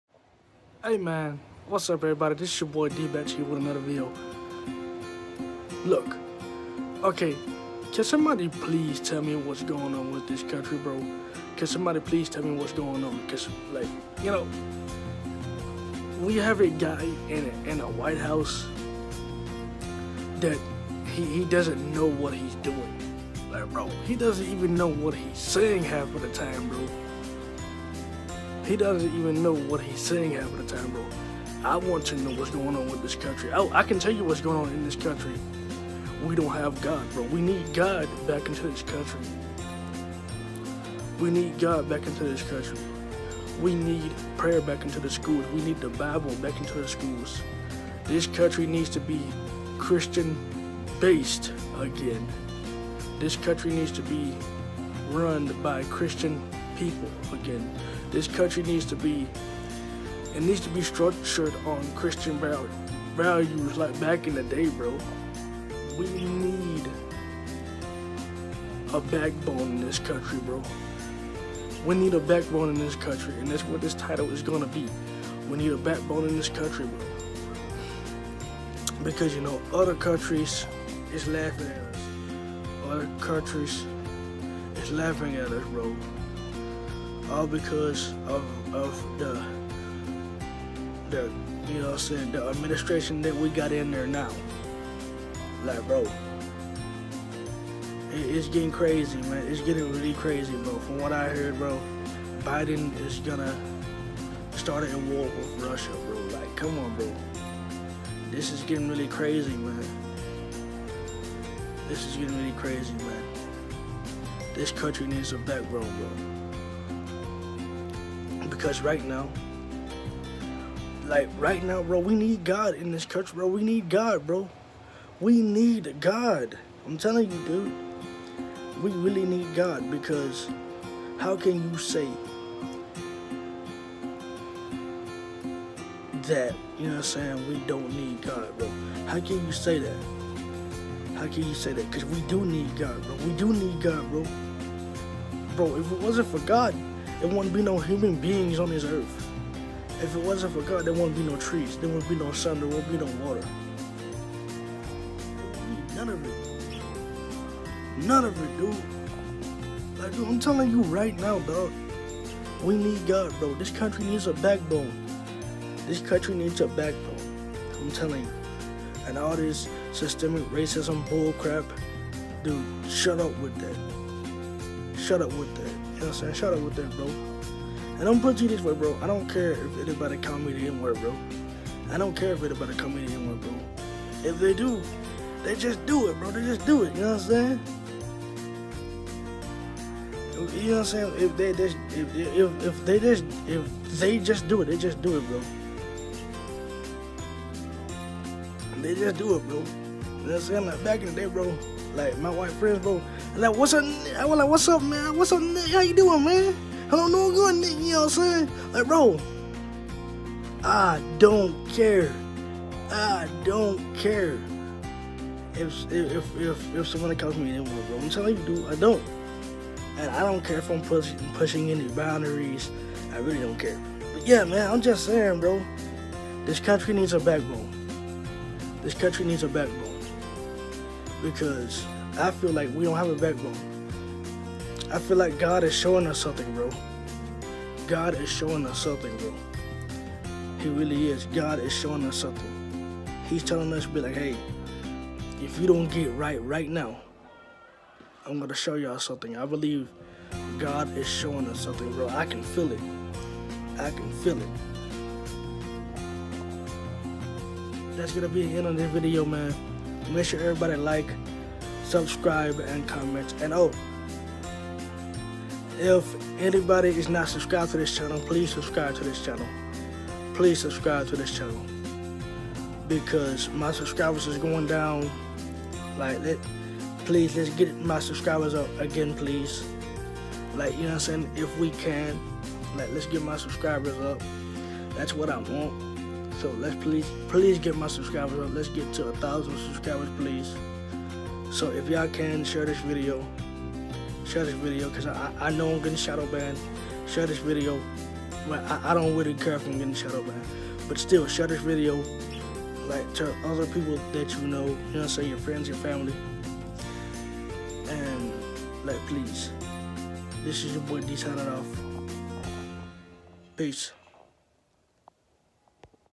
What’s going on with this Country￼
What’s Up everybody I want to know what’s going on in this country we need God back into this country We need the Bible back into the schools Make sure everybody like subscribe and comment BlindSquad￼ BlindSquadRecords￼What’sGoingOnInThisCountry￼￼￼